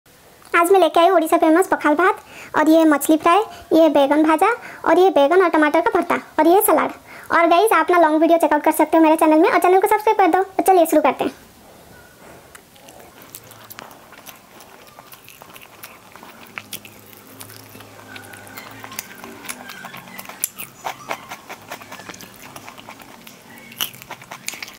Eating Odisha famous Pakhala Bhat , Fish fry , Salad ,Begun Bhaja
ASMR Mukbang : Odisha  famous Pakhala Bhat( Water Rice / Panta Bhat)  , Fish fry , Salad ,Begun  Bhaja.